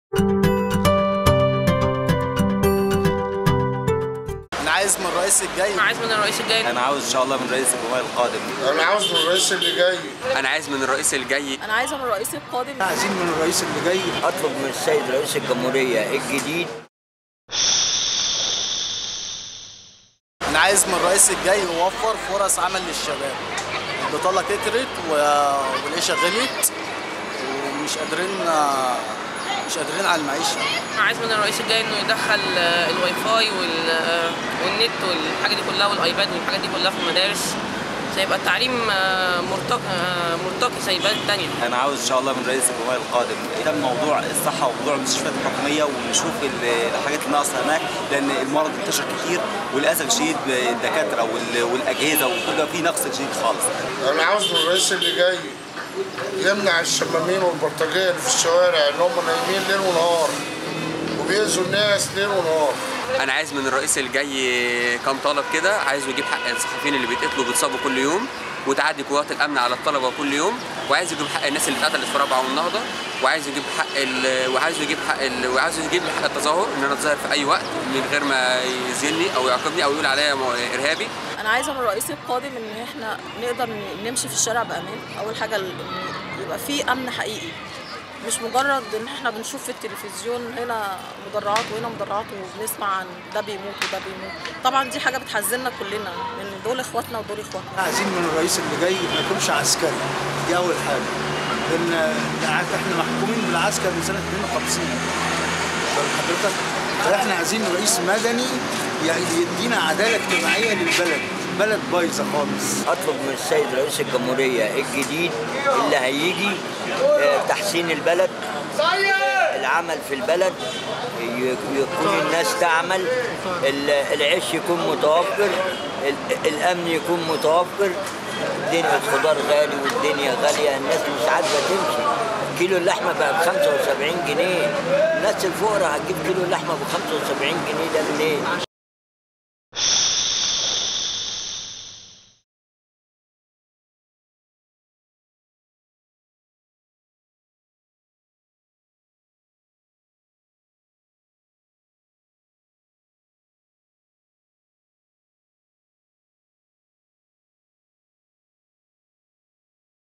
أنا عايز من الرئيس...
في استطلاع لطموحات بعض المصريين في الرئىس القادم وما سيقدمه للبلاد في المرحلة القادمة، كانت هذه اللقاءات مع شرائح مختلفة من المصريين، حيث طالبوا فيها الرئىس بتحسين الأوضاع الاجتماعية والصحية والأمنية والنظر لمنظومة التعليم بنظرة متطورة وتحسين أوضاه الشباب بتوفير فرض للعمل بما يناسب امكانياتهم .

تقرير: عمر ساهر، معتز موسي، محمد صبري، محمد فتحي
مونتاج: أحمد متولي
إخراج: إيناس مرزوق